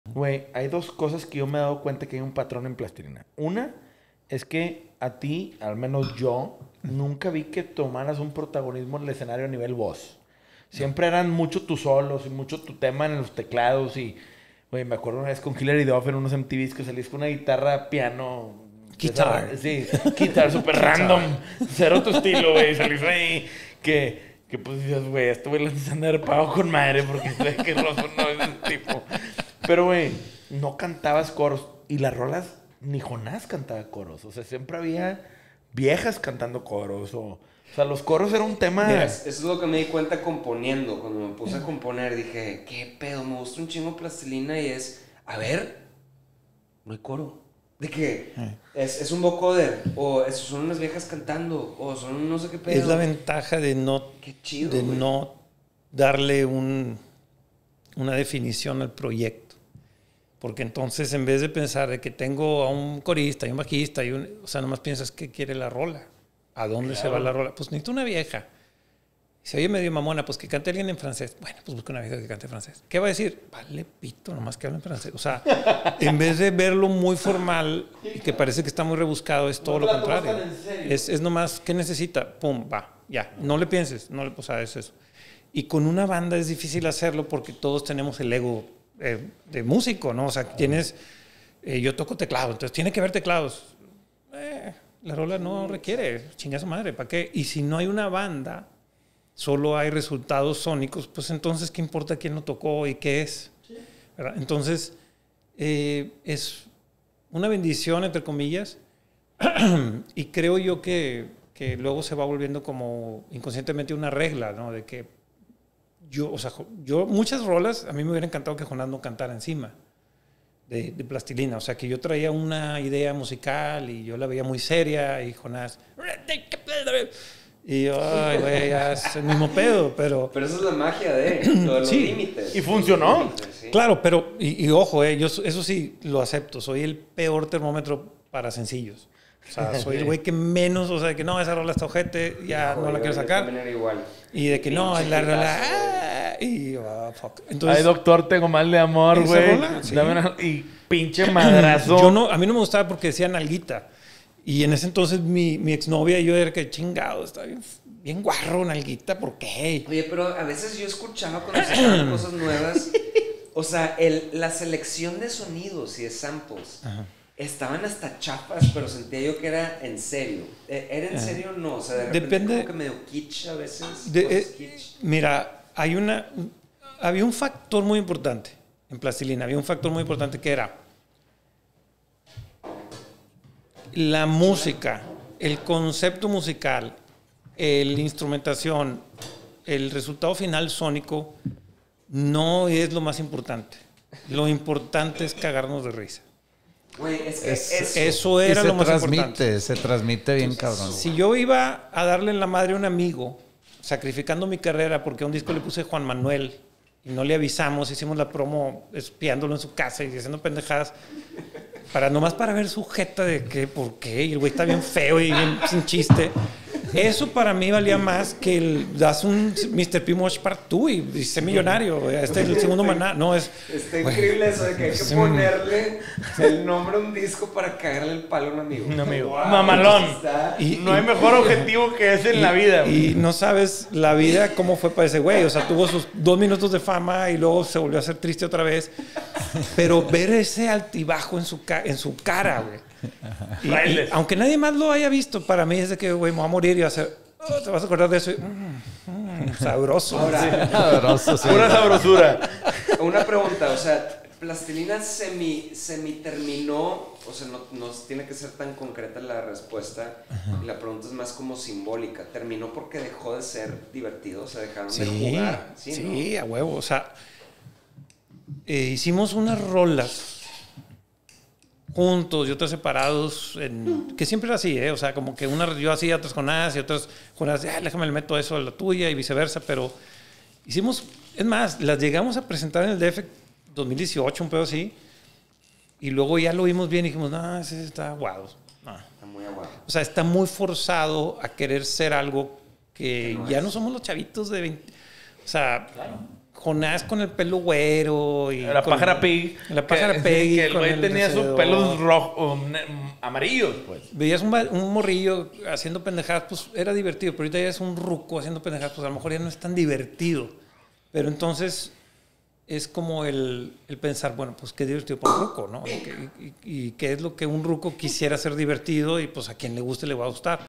Cag*rse de risa... Y cuándo Plastilina dejó de ser divertido
Bienvenidos al nuevo episodio de Sellout el podcast de Negro Pasión, ya les hicimos caso ahora es a color jajaja! En este nuestro episodio #52  platicamos con un grande de la musica regiomontana Alejandro Rosso, integrante de la famosa Plastilina Mosh, tecladista, productor y tomador profesional de vino. Fue una platica entre amigos muy chida en donde nos echamos cotorreo de todo un poco.

Episodio 052 de SELLOUT by Negro Pasión.